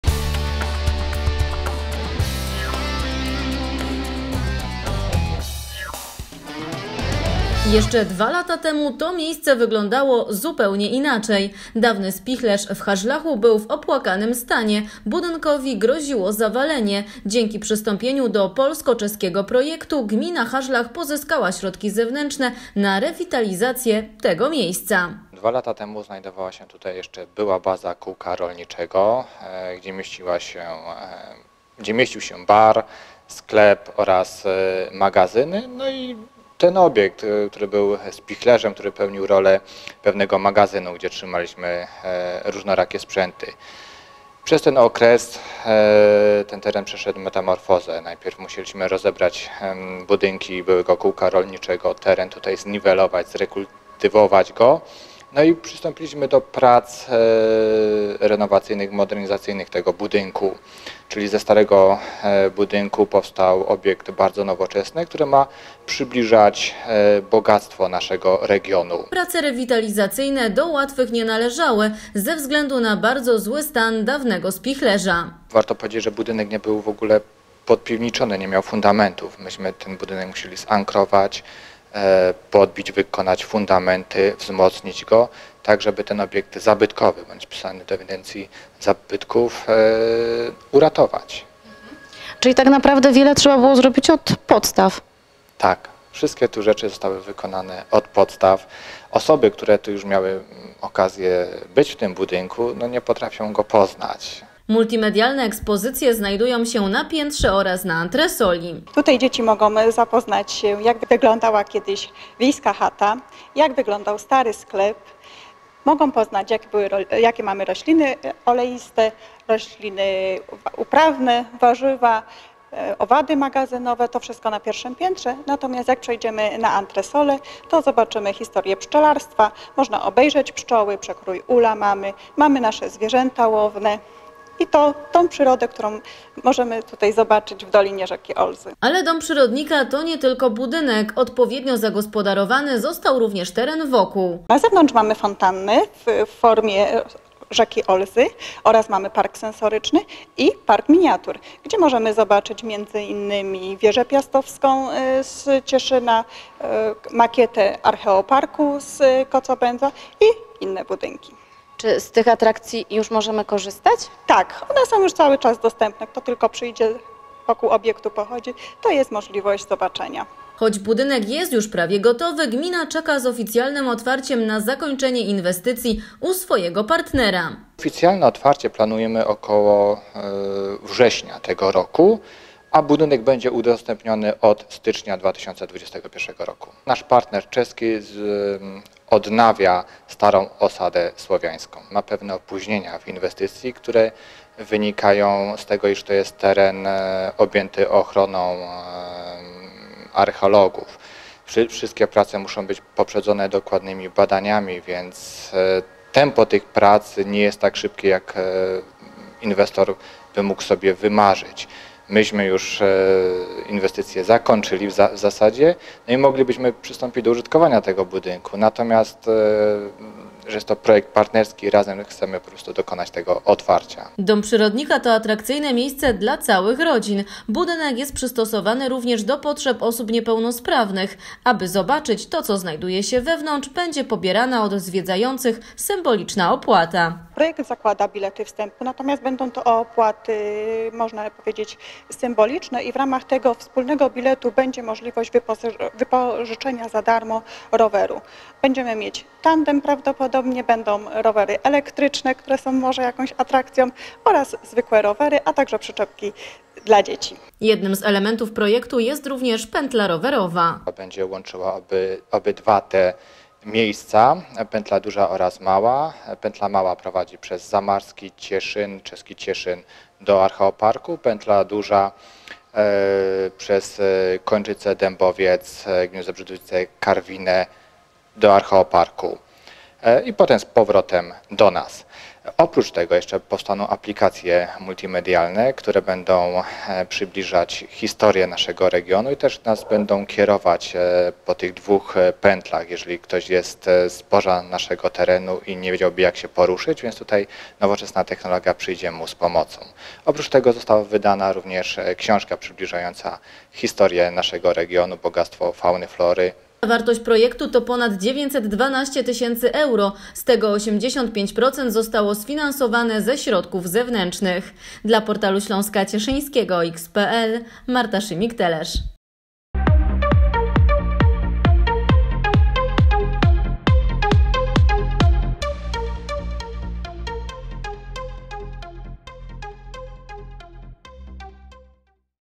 Dom Przyrodnika od środka - zobacz jak wygląda
Dom Przyrodnika w Hażlachu jest już prawie gotowy. Jednak na oficjalne otwarcie trzeba będzie jeszcze poczekać. Zaplanowano je we wrześniu tego roku. Dlaczego Dom Przyrodnika swoje podwoje otworzy dopiero jesienią o tym w materiale filmowym.